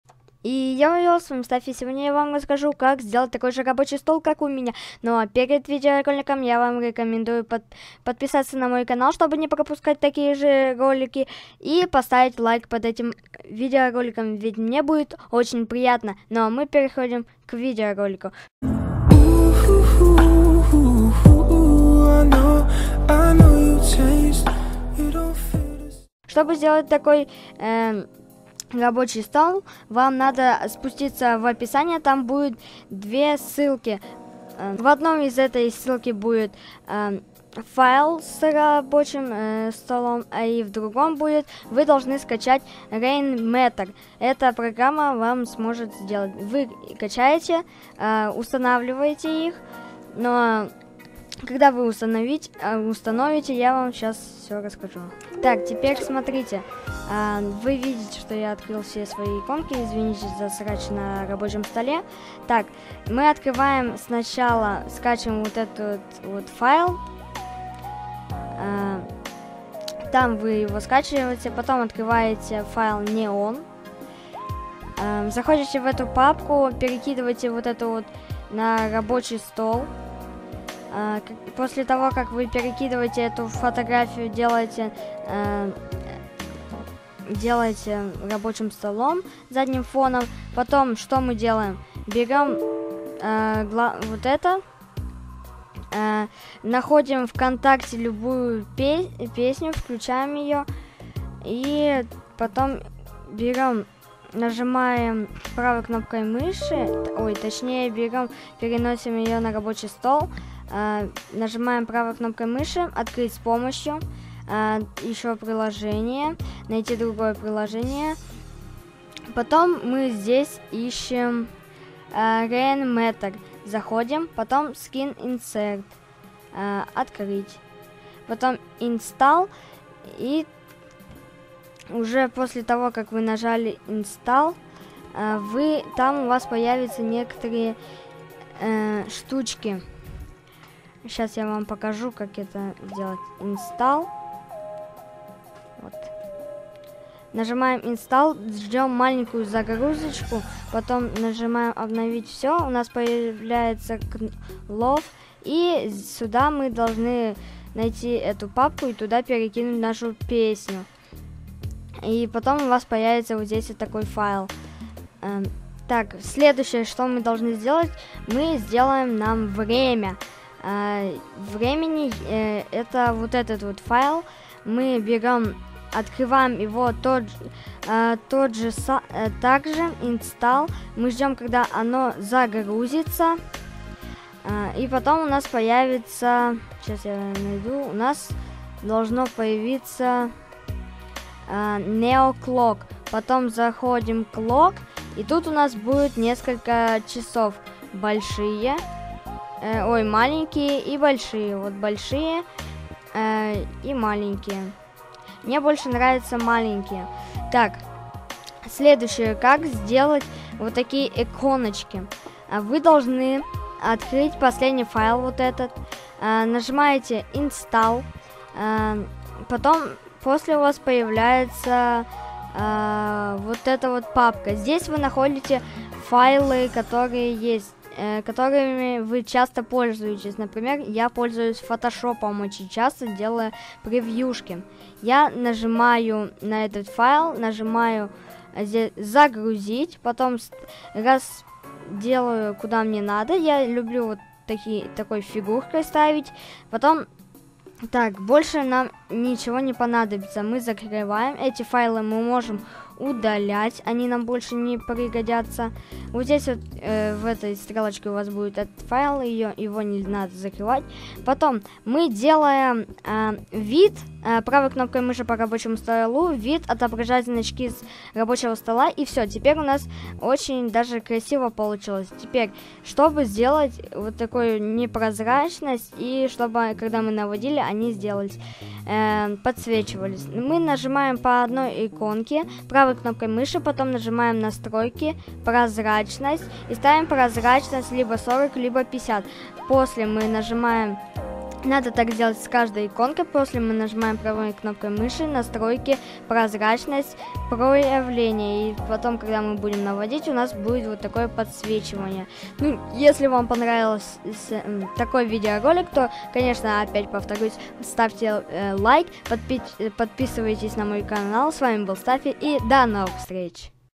КАК СДЕЛАТЬ КРАСИВЫЙ РАБОЧИЙ СТОЛ // НА WINDOWS 10
В этом ролике я расскажу как украсить ваш рабочий стол, и сделать его не много красивее, делается это очень легко.
❤️ Мой вк : Не сказю
💜 Моя инста : Не сказю

🌈 Ненужные теги : Как украсить рабочий стол, красивый рабочий стол windows 10